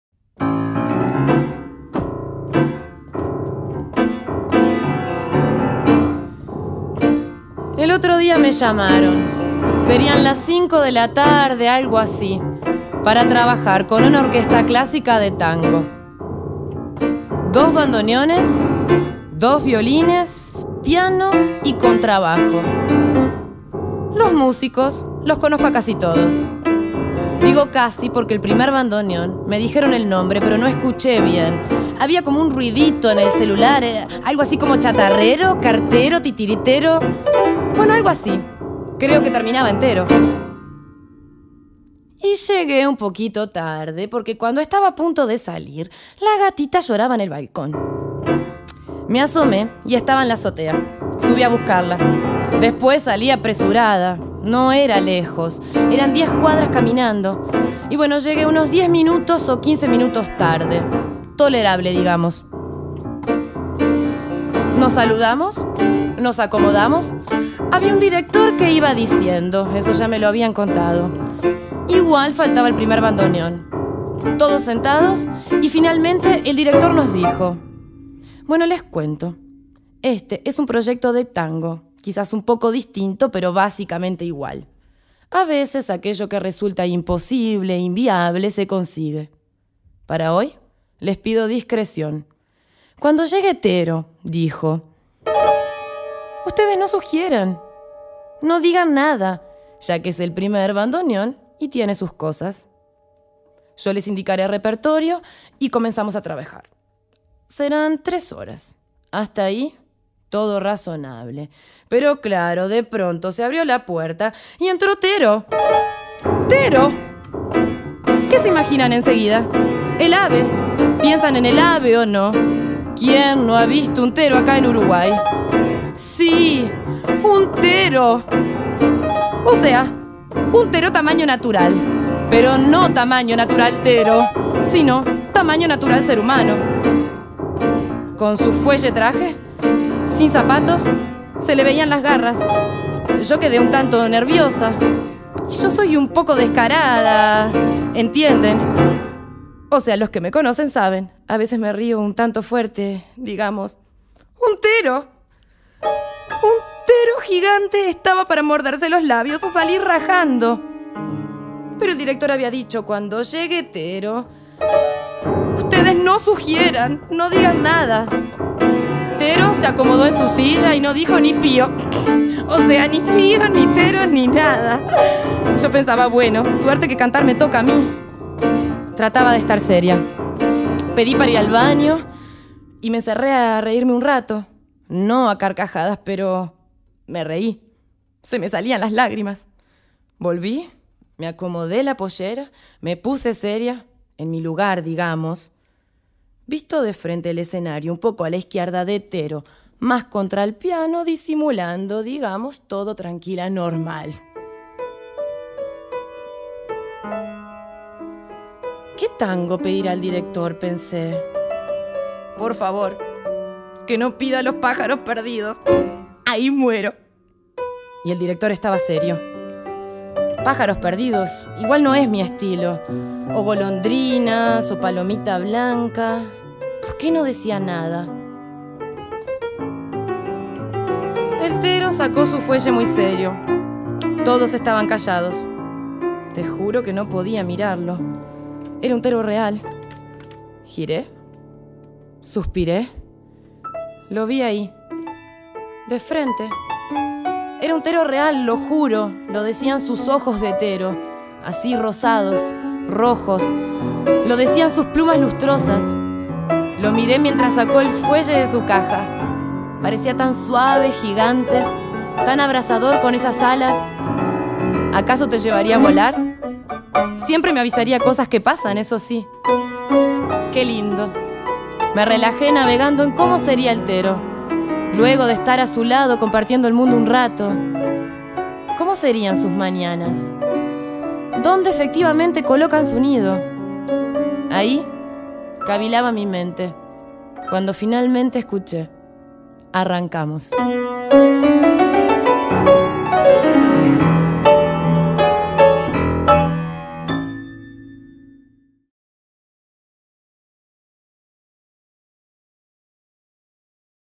Amor de tango y tero.
de Tamara del Castillo
lectura Maggy Wohler
piano Javier Toledo